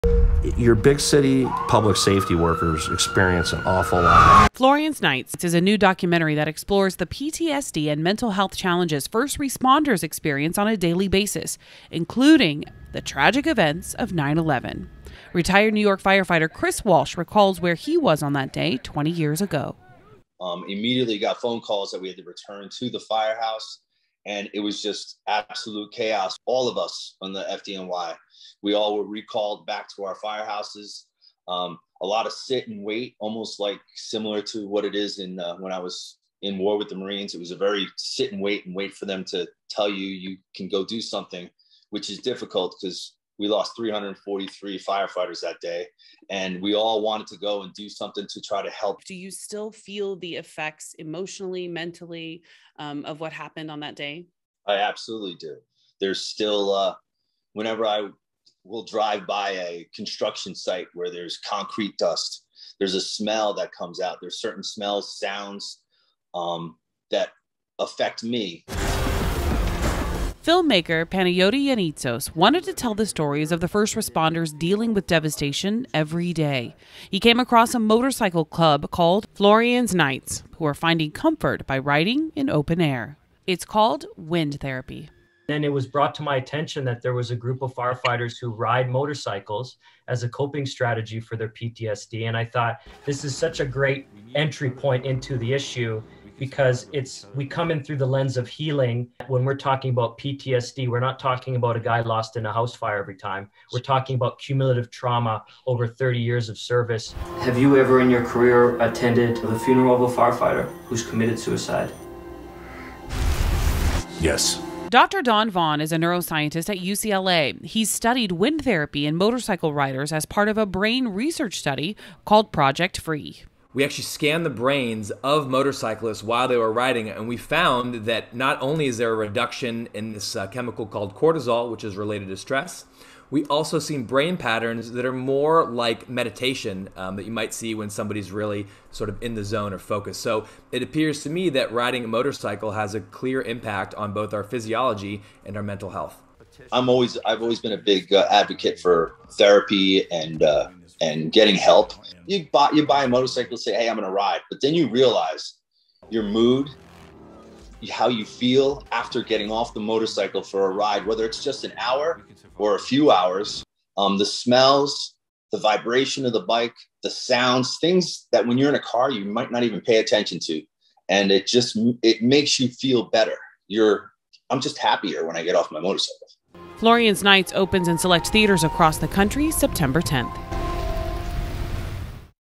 ‘Florian's Knights' Explores How Firefighters Ride Motorcycles to Treat PTSD
From the impact of 9/11, to the front lines of the opioid crisis, "Florian's Knights" explores what firefighters are exposed to on a daily basis and how they cope by riding motorcycles. NBC entertainment reporter Heather Brooker sat down with filmmakers Panayioti Yannitsos to get details. The film opens in select theaters Sept. 10 and digital platforms on November 23.

About NBCLA